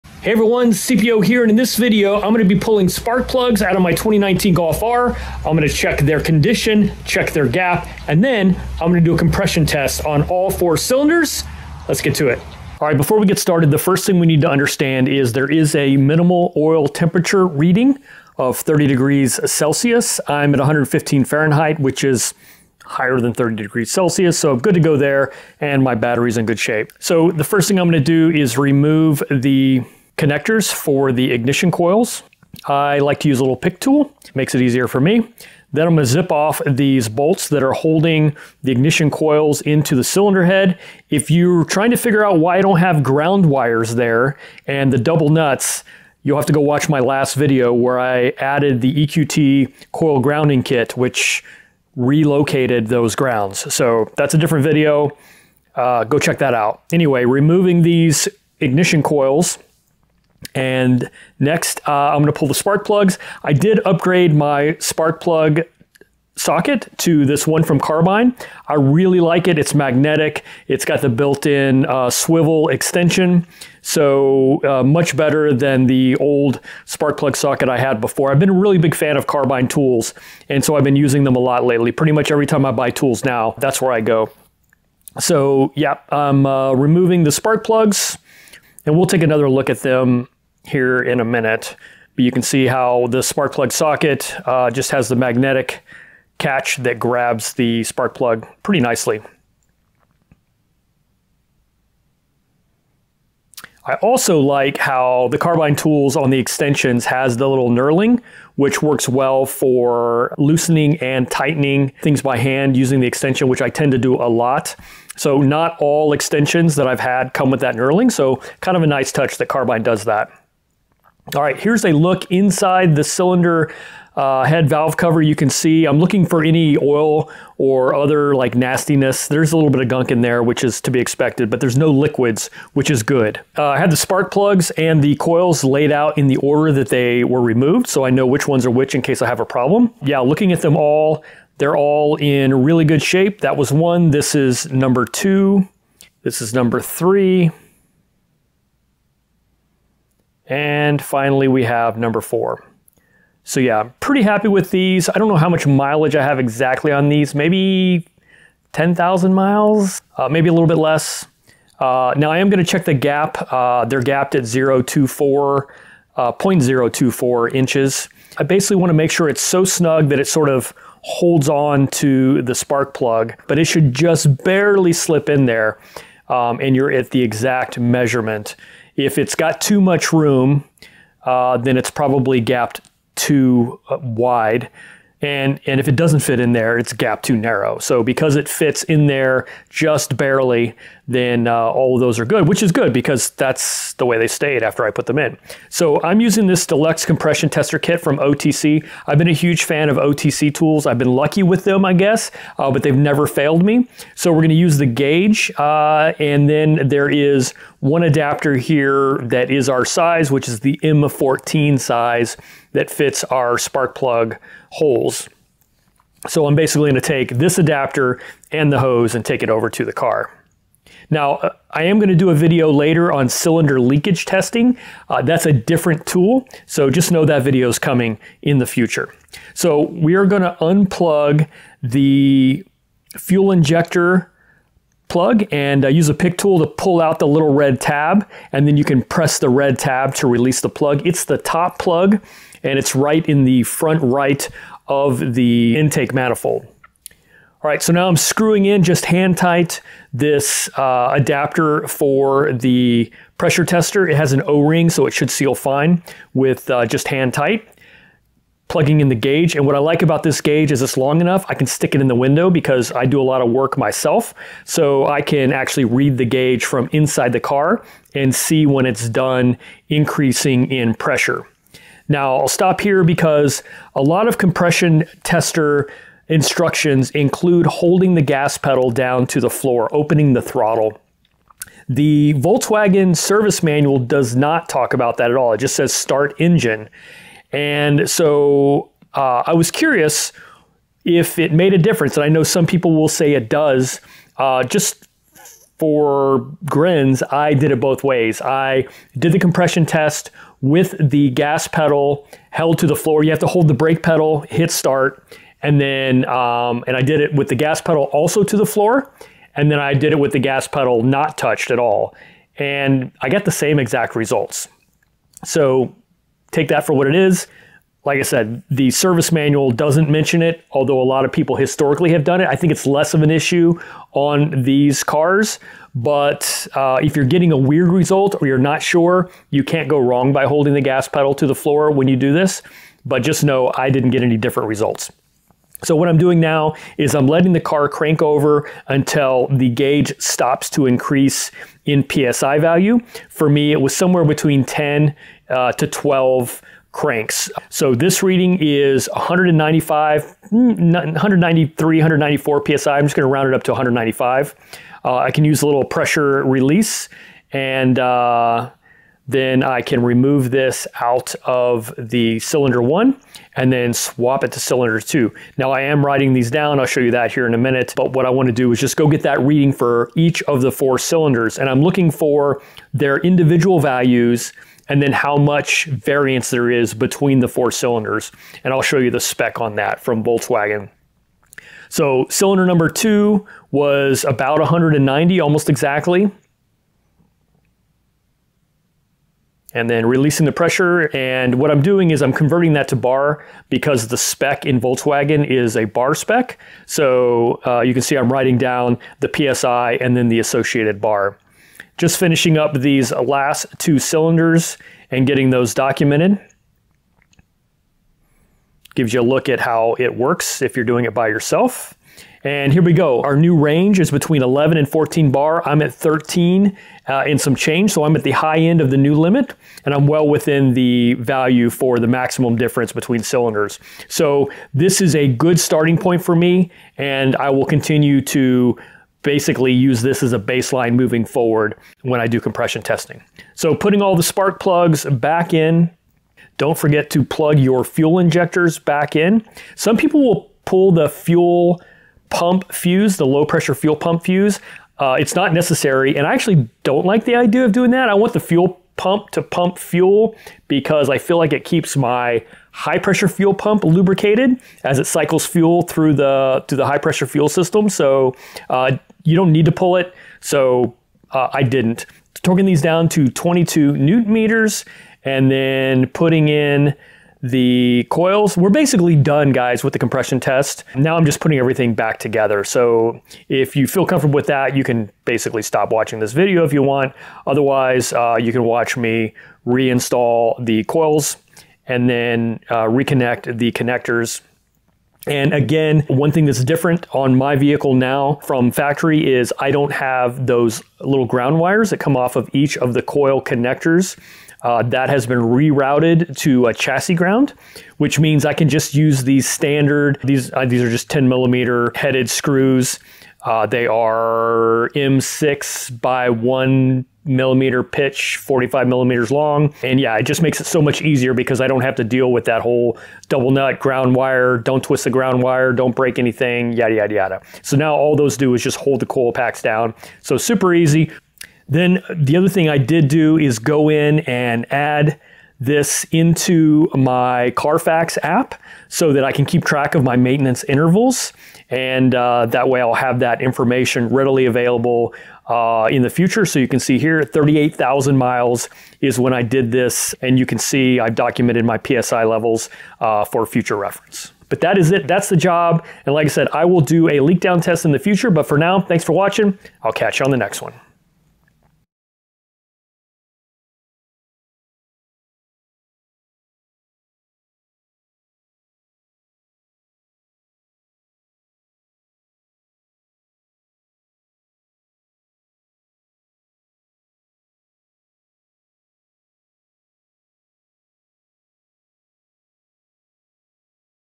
MK7.5 Golf R Compression Test and Spark Plug Check
Doing a compression test and checking my plugs since I have been running EQT Stage 2 E85 for a while now.

Here are some links for you.

Compression Tester
Leak Down Test Kit
Carbyne Spark Plug Socket
Carbyne Tools Store